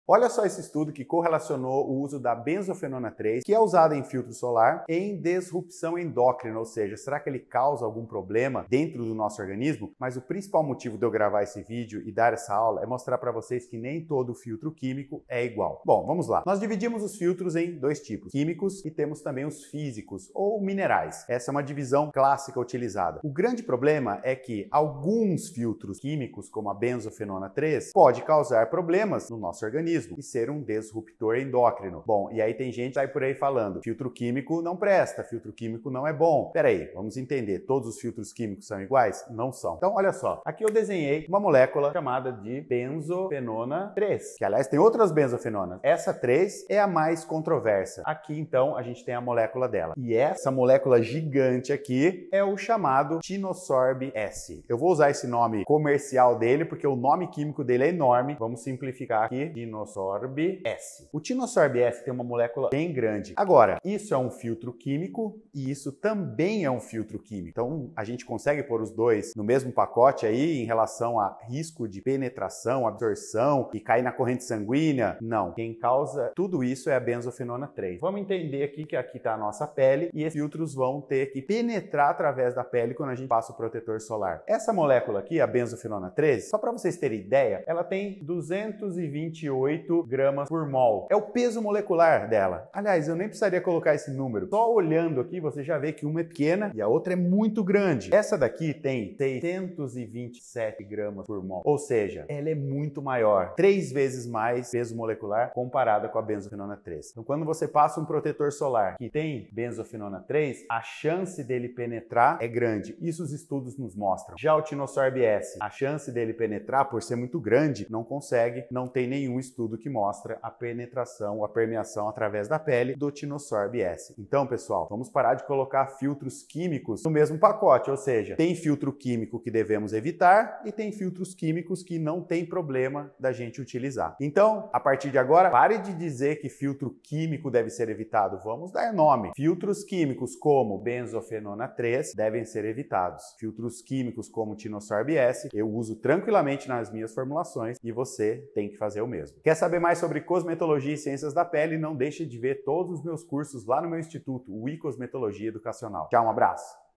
Filtro solar causa câncer?
🟧 tá na hora de parar de falarmos de forma genérica dos filtros químicos.

🟨 Essa classe de substâncias pode ser dividia em 2 categorias:
1. moléculas lipofílicas pequenas que penetram e podem ser absorvidas através da corrente sanguínea
2. moléculas lipofílicas grande que não conseguem penetrar

Porque essa divisão? nessa aula eu explico que devido ao peso molecular, não há indícios de permeação de moléculas grandes.

Claro que se vcs tiverem novas evidências, por favor me mandem!

abraços